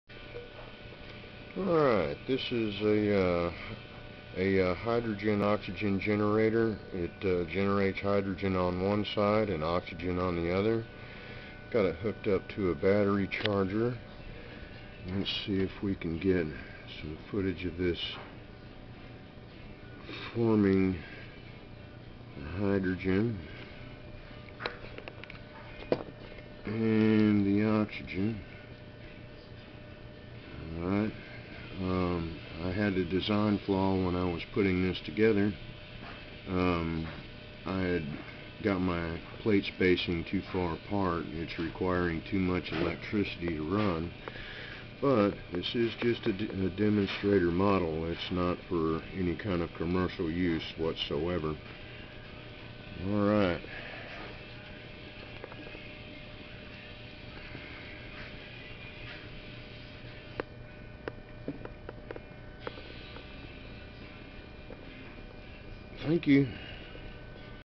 Hydrogen - Oxygen electrolizer, teaching aid & demonstration unit.
This is to demonstrate that hydrogen and oxygen can be generated at the same time and in the same device.